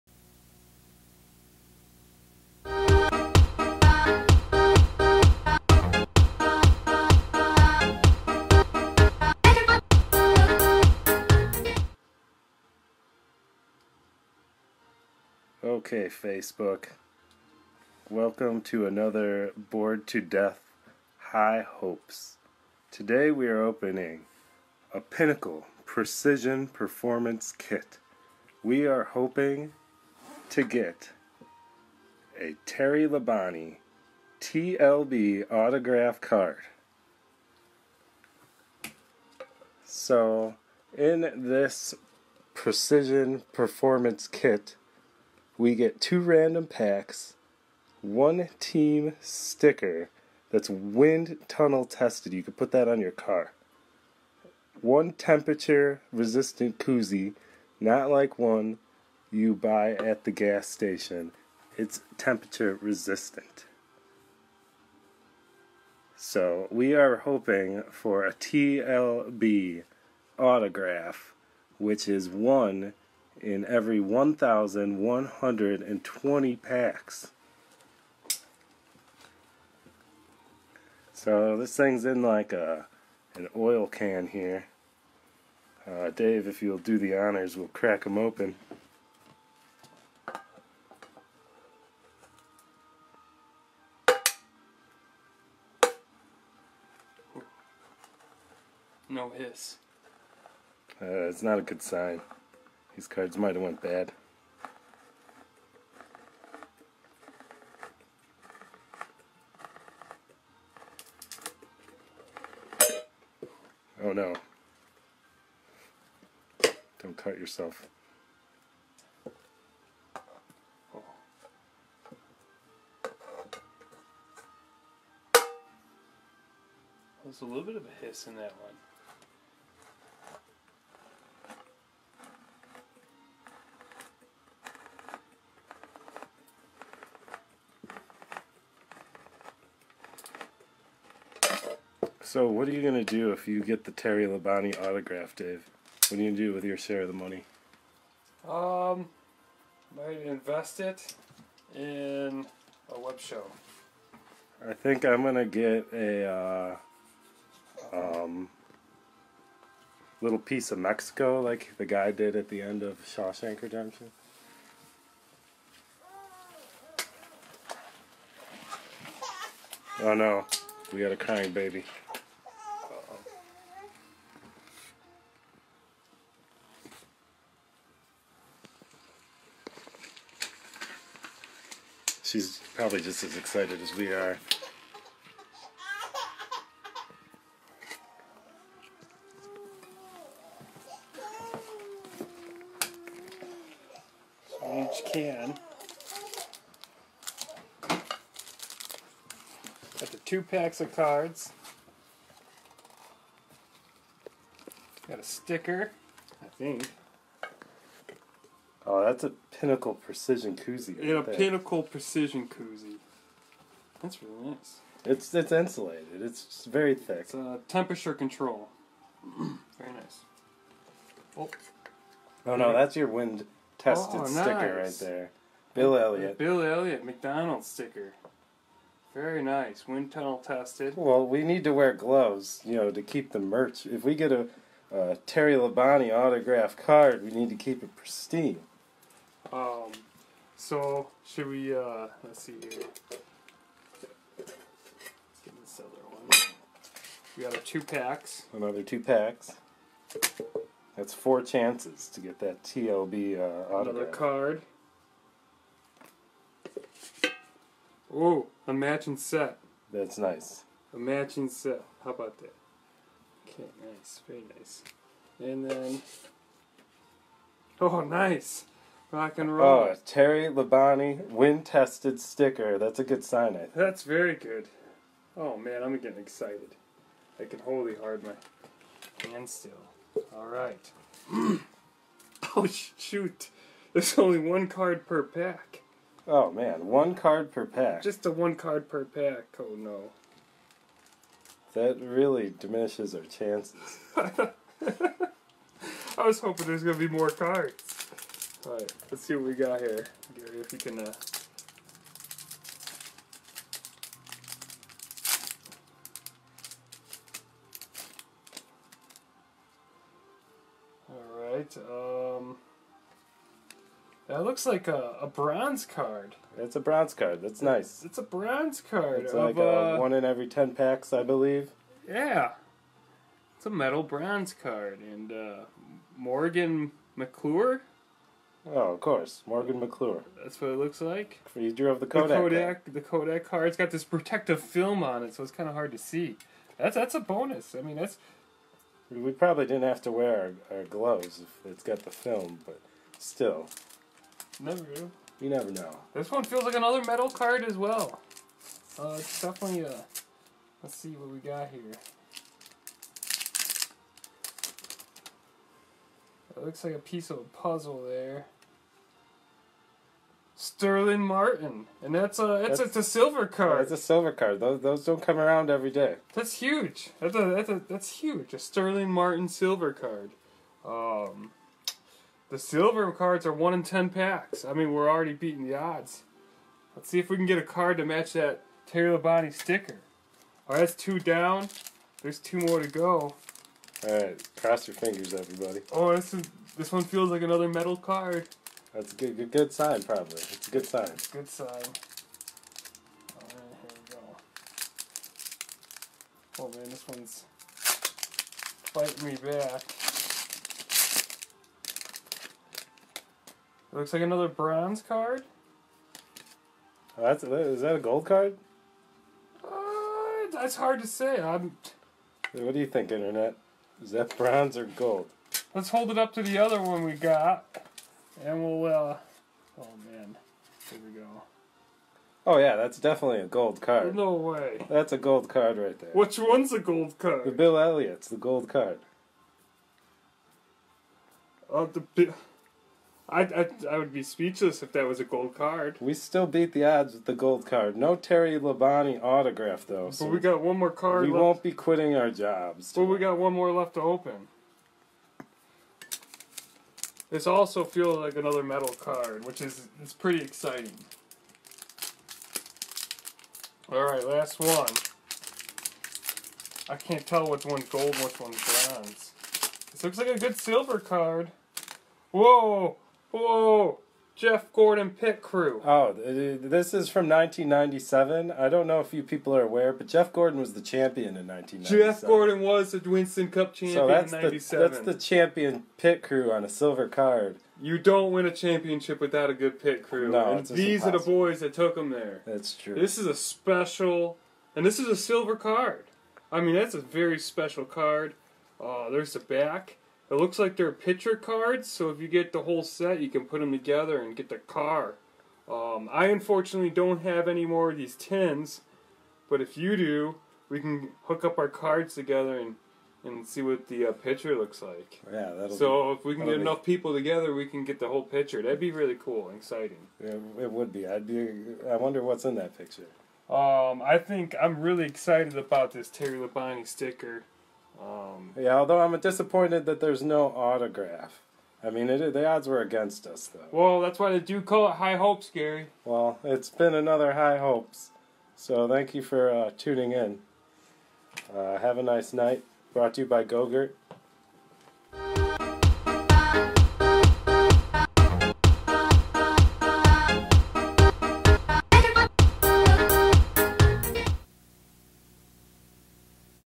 High Hopes Opens: Vintage 1997 Pinnacle Precision Performance Kit Terry Labonte Can
Today we go back in time to our pilot episode. Marvel at how little we’ve learned over the years as we open Vintage 1997 Pinnacle Precision Performance Kit Terry Labonte Cans!
Episode: 0